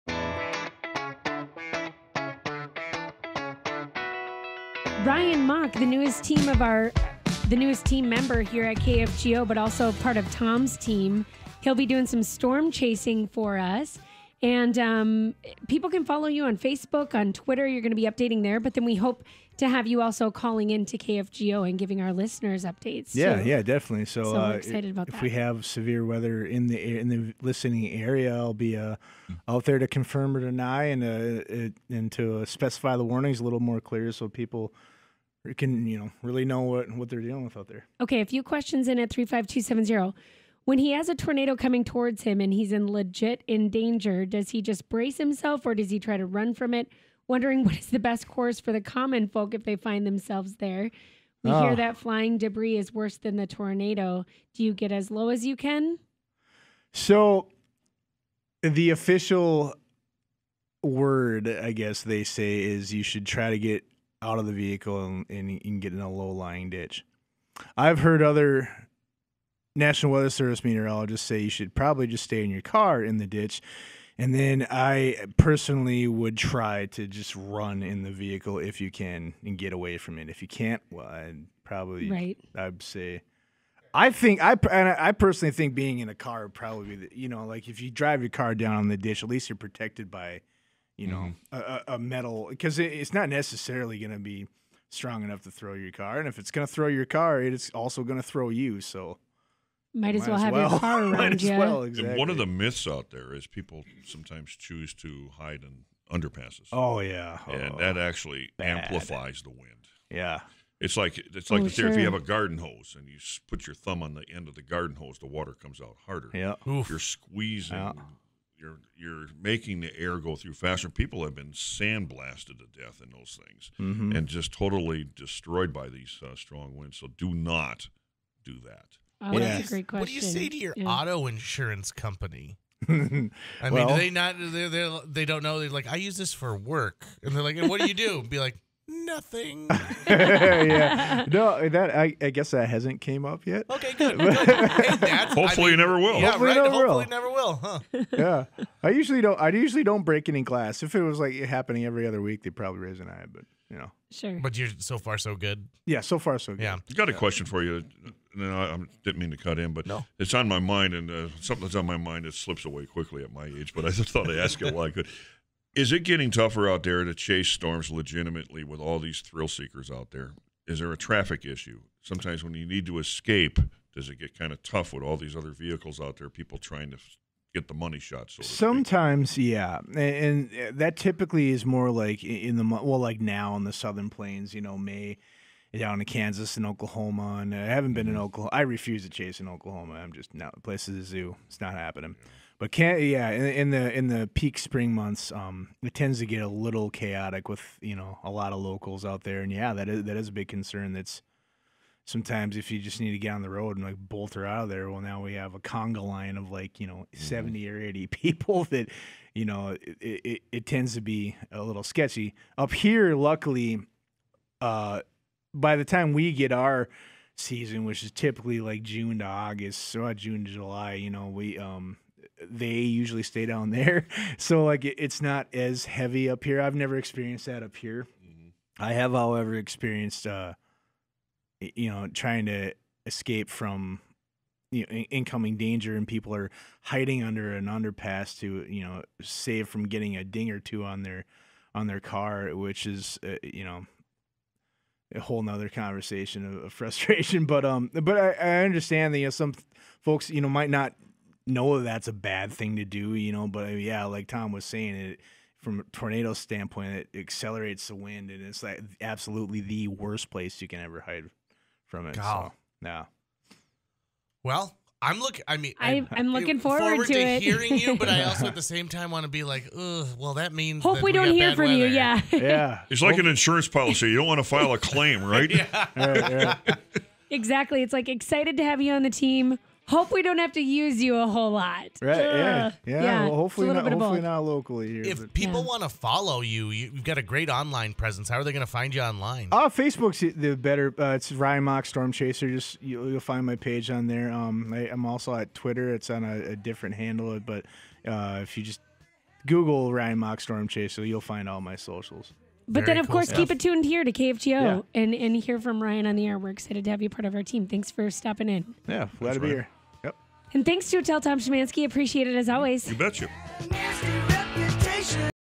Meet KFGO's Storm Chaser Ryan Mauk
KFGO is proud to announce the newest member of the team! Ryan Mauk! He'll be chasing storms for you this summer storm season!